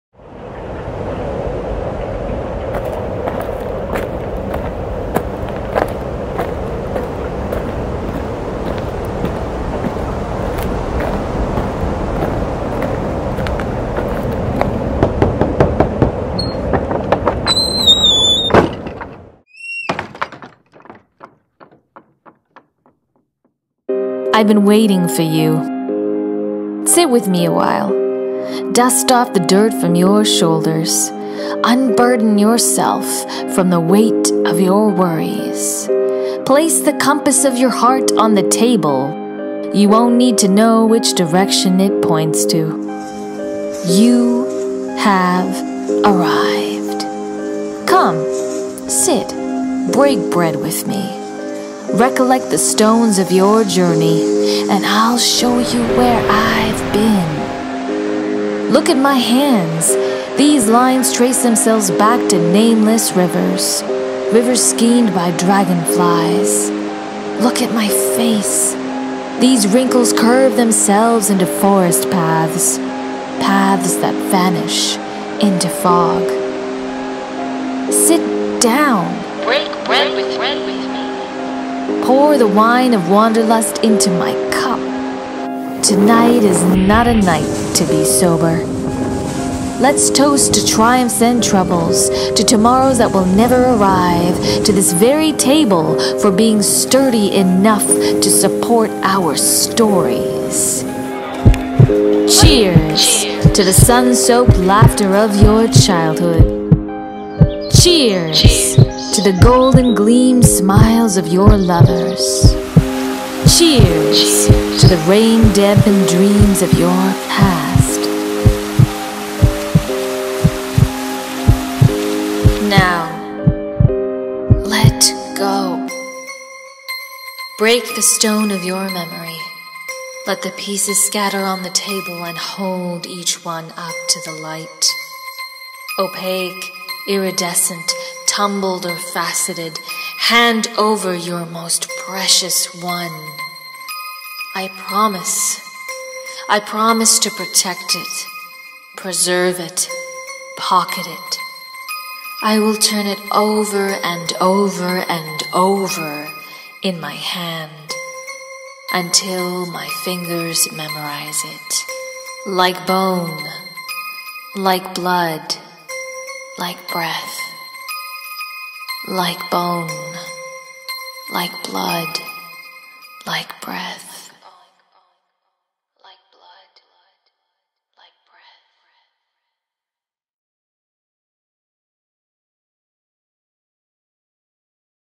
Breaking Bread
Original poem and recitation by Sheniz Janmohamed.
Audio recording, footage, editing and video by Sheniz Janmohamed

Sit with me a while,
dust off the dirt from your shoulders.
Unburden yourself from the weight
of your worries.

Place the compass of your heart
on the table.
You won’t need to know which direction
it points to.

You have arrived.

Come, sit.
Break bread with me.
Re-collect the stones of your journey
and I’ll show you where I’ve been.

Look at my hands. These lines trace themselves back to nameless rivers,
rivers skeined by dragonflies.
Look at my face. These wrinkles curve themselves into forest paths,
paths that vanish into fog.

Sit down.
Pour the wine of wanderlust into my cup,
Tonight is not a night to be sober.

Let’s toast to triumphs and troubles,
to tomorrows that will never arrive, to
this very table, for being sturdy enough to
support our stories.

Cheers!

Cheers! to the sun-soaked laughter of your childhood.
Cheers! to the golden-gleamed smiles of your lovers.
Cheers! to the rain-dampened dreams of your past.

Now, let go.

Break the stone of your memory
Let the pieces scatter on the table
and hold each one up to the light.

Opaque, iridescent, tumbled, or faceted,
hand over your most precious one.

I promise to protect it, preserve it, pocket it.

I will turn it over and over
in my hand,
until my fingers memorize it.

Like bone,
Like blood,
Like breath.